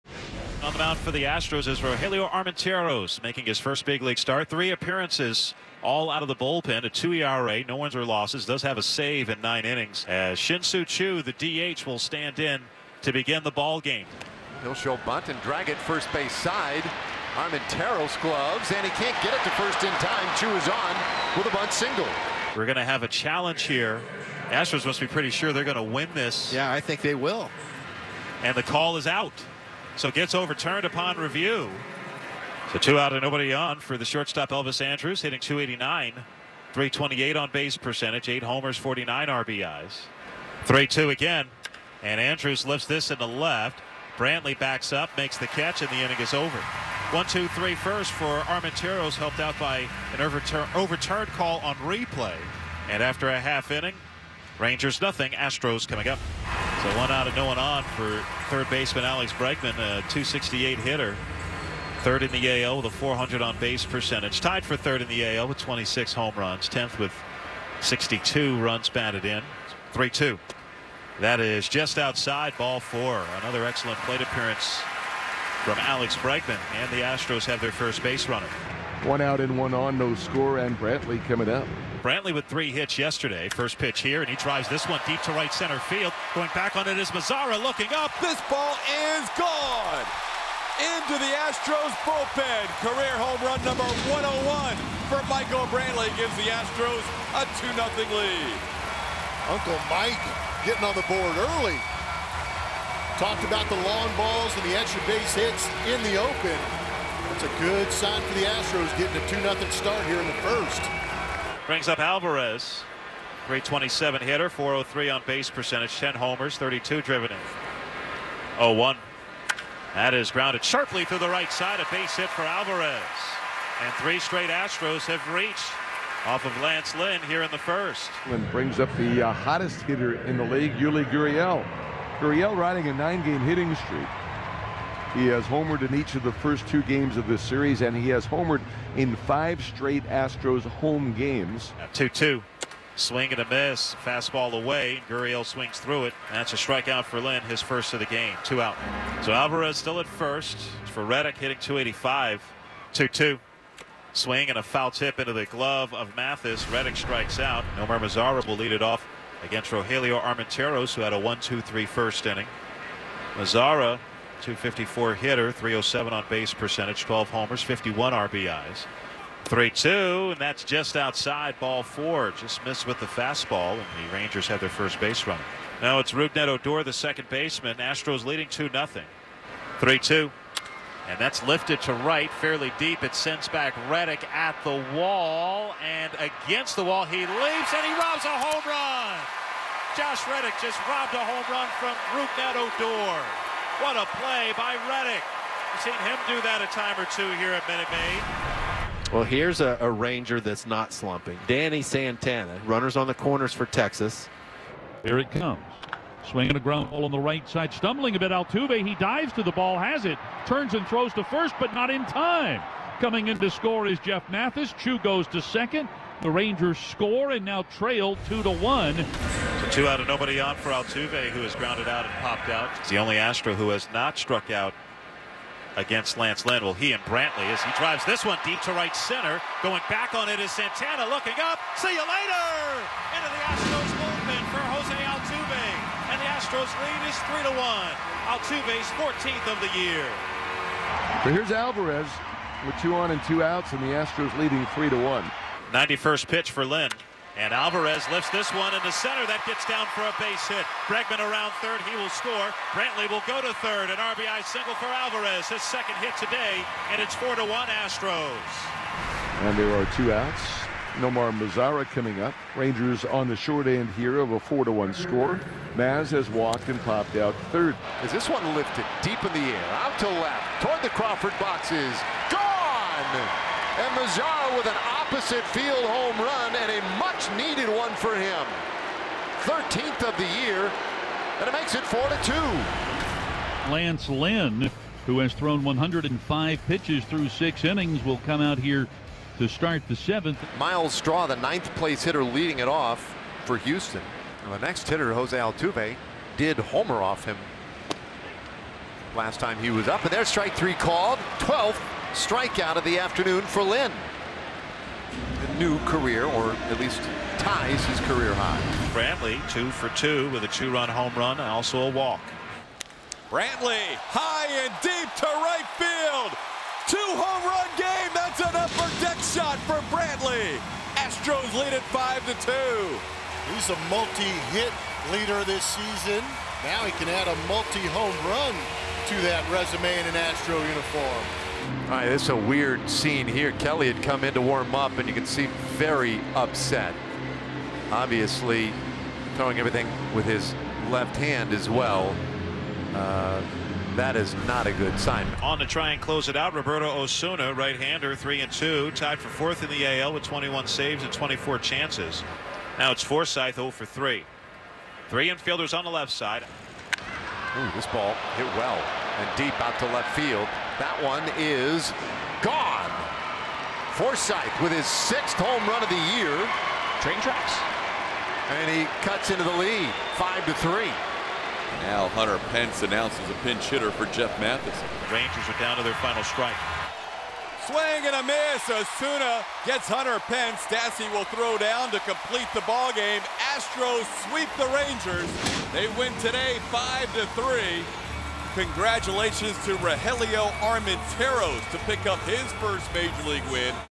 Brantley homers twice, Astros sweep Rangers | Rangers-Astros Game Highlights 7/21/19
Condensed Game: Michael Brantley clubbed a pair of home runs while Jose Altuve added a solo smash of his own as the Astros swept the Rangers